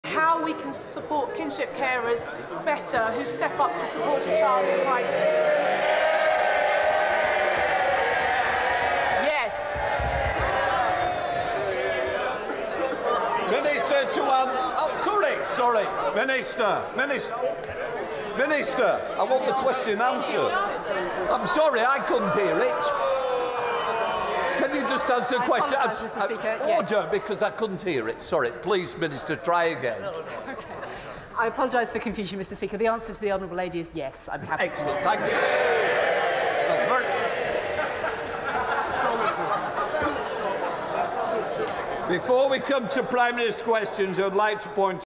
Boris Johnson jeered and booed as he enters Commons for PMQs
PMQs LIVE: Boris Johnson jeered and booed as he enters Commons for PMQs. Boris Johnson today faces brutal PMQs after resignation as Tory leadership race heats up. Today's Prime Minister's Questions comes as the Tory leadership race heats up, with the first ballot of Tory MPs will take place between 1.30pm to 3.30pm, with a result to be announced later in the day. Any candidates who receive fewer than 30 votes in this first ballot will be automatically eliminated. If all candidates meet the 30-vote threshold, then the candidate with the lowest number of votes will be knocked out the contest.